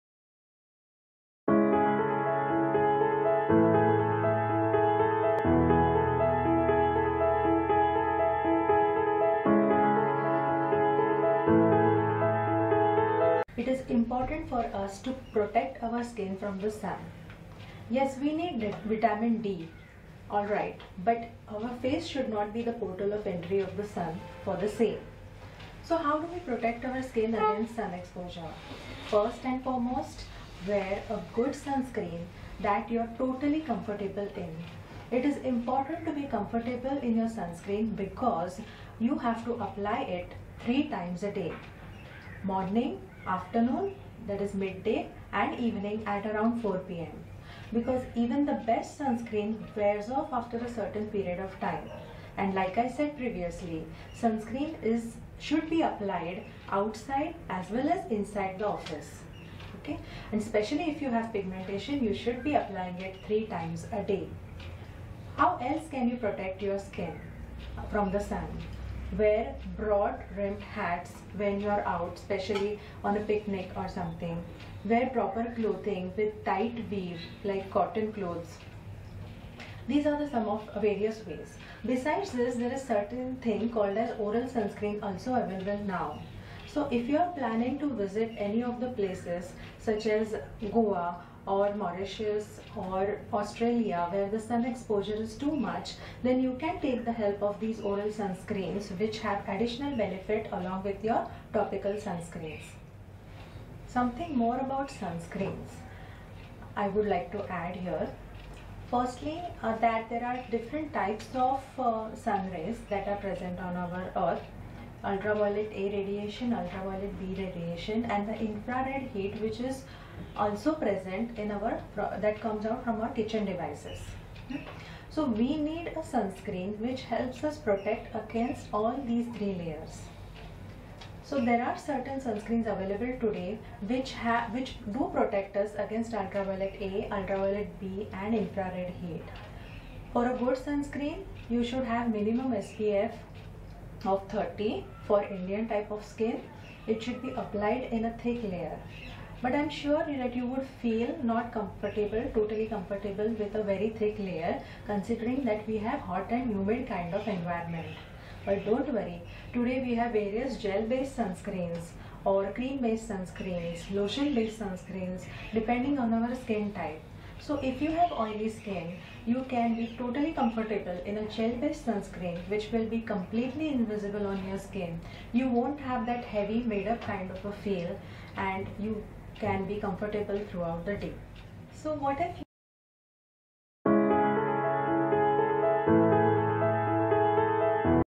National Sun Screen Day message by Dr Rupali Pavaskar
On occasion of World Sun Screen Day , Let's Listen to message from Dr.Rupali Pavaskar